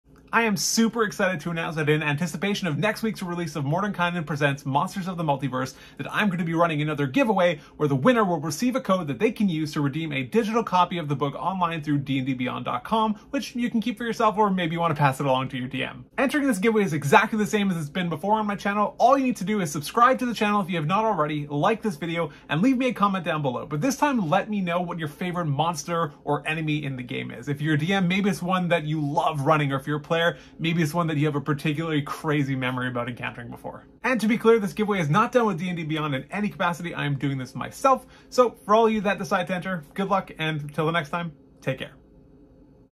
GIVEAWAY! Mordenkainen's Monsters of the Multiverse | Insight Check
In this video I am giving away a digital copy of Mordenkainen Presents: Monsters of the Multiverse through DNDBeyond.com.

This is not a sponsored video and DNDBeyond has no participation in this giveaway.

Winners will be selected Tuesday, January 25, 2022 by selecting a random number from the total comments posted. Multiple entries are ignored in determining the winner.